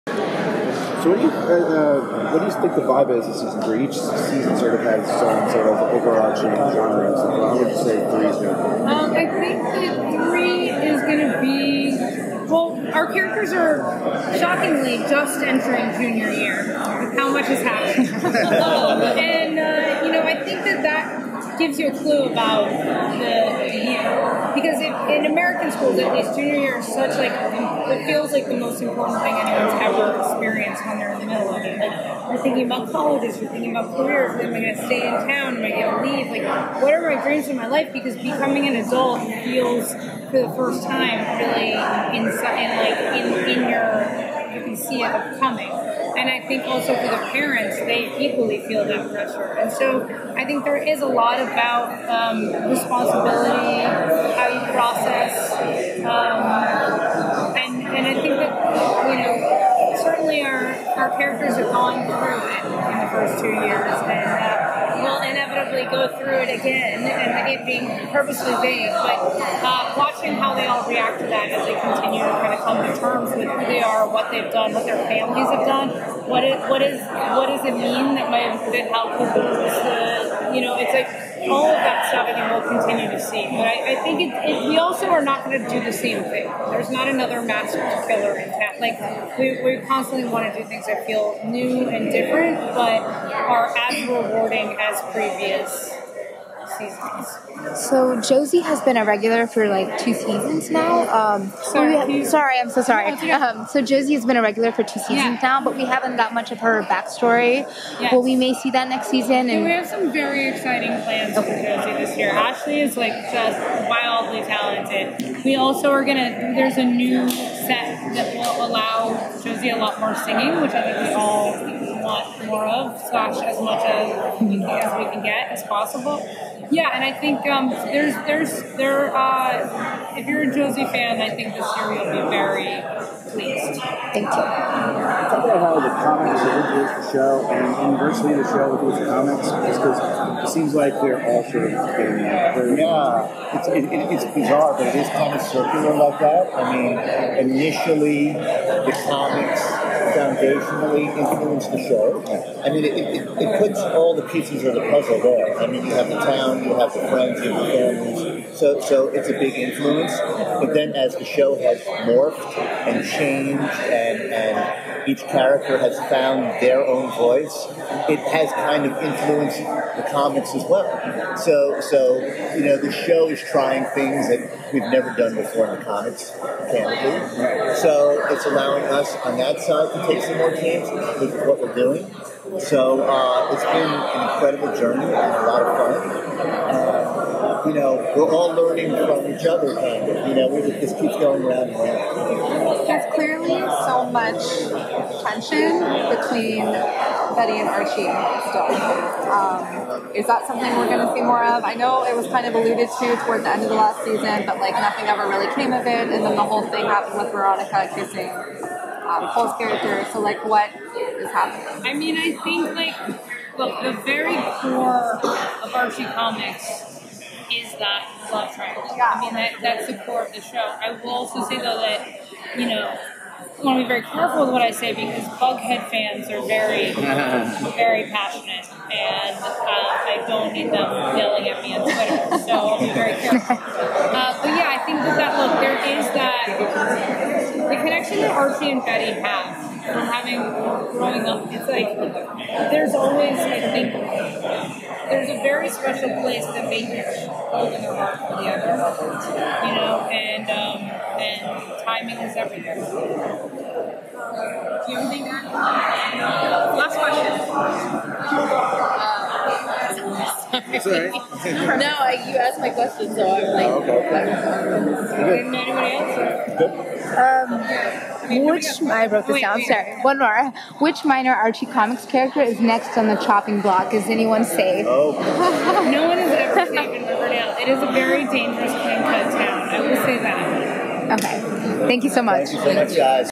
SDCC 2018: Riverdale - Jon Goldwater & Sarah Schechter
Riverdale EPs Jon Goldwater & Sarah Schechter talk about next season's tone, the rise of Josie McCoy and the potential of Barchie for S3.